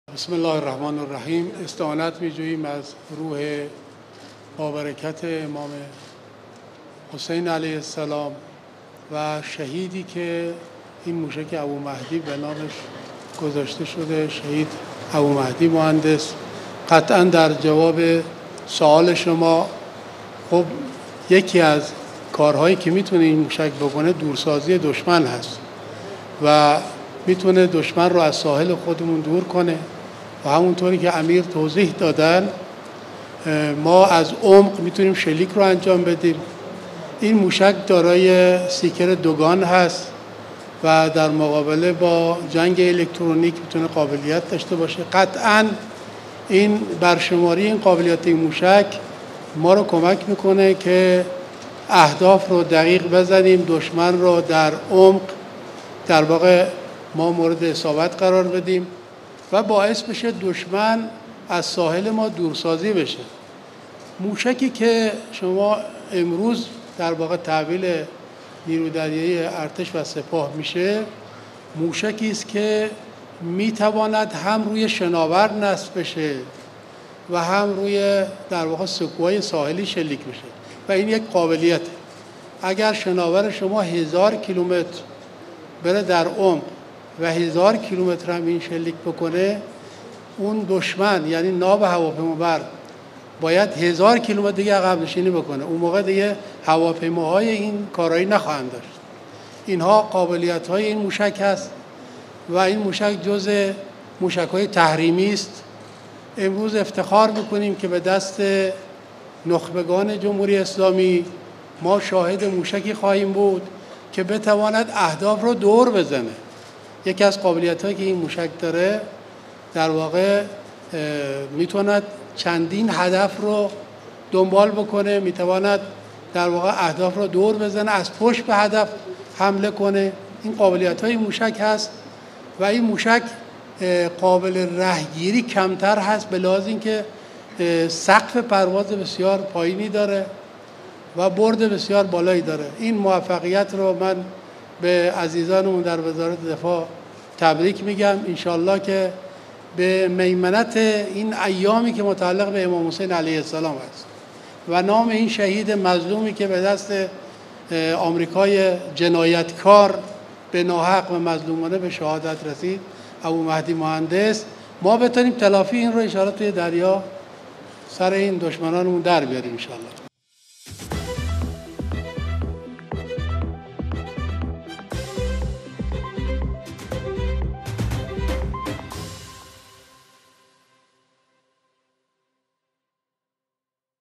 Iran's Abu Mahdi missile, a game-changer in countering US aircraft carriers: Cmmdr.
Brigadier General Alireza Tangsiri, commander of the Iran Navy, underscored the pivotal role the Abu Mahdi missile will play in bolstering the country's defense capabilities.